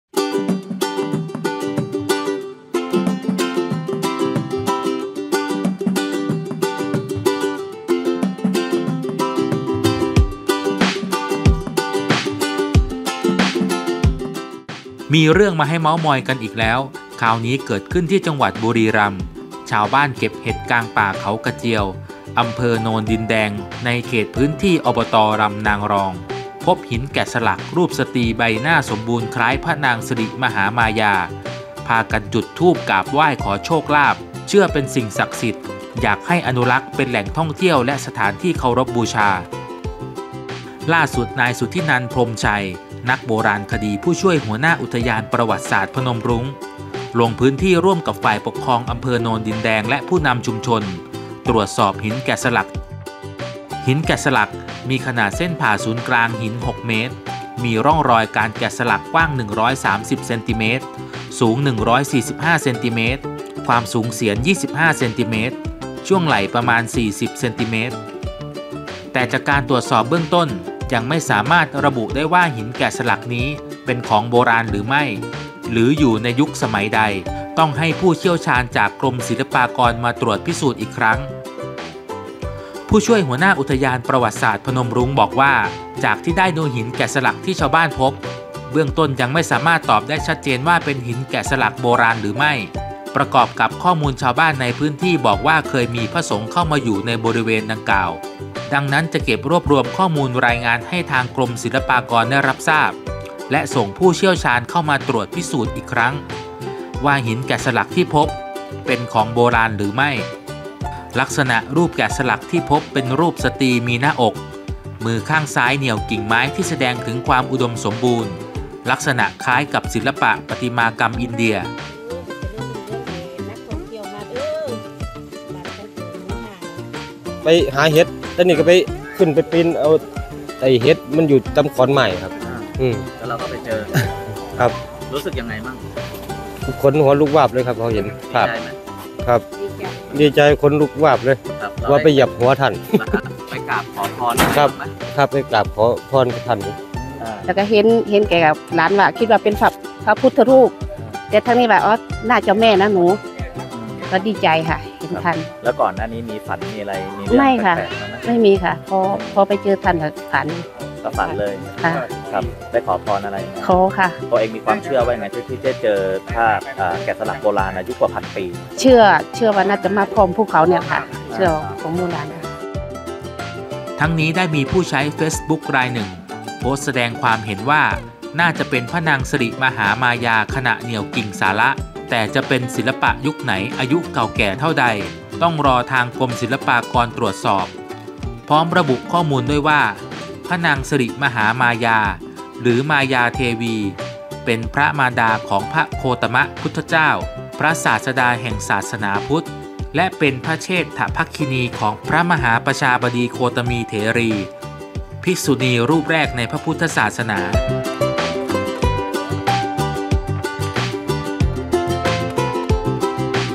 แห่กราบ! หินแกะสลัก รูปคล้ายพระนางสิริมหามายากลางป่าเขากระเจียว : เรื่องเด่นทั่วไทย
ชาวบ้านเข้าป่าเก็บเห็ดกลางป่าเขากระเจียว อำเภอโนนดินแดง   อบต.ลำนางรอง  พบหินแกะสลักรูปสตรีใบหน้าสมบูรณ์คล้าย “พระนางสิริมหามายา”พากันจุดธูปกราบไหว้าขอโชคลาภ เชื่อเป็นสิ่งศักดิ์ สิทธิ์   อยากให้อนุรักษ์เป็นแหล่งท่องเที่ยวและสถานที่เคารพบูชา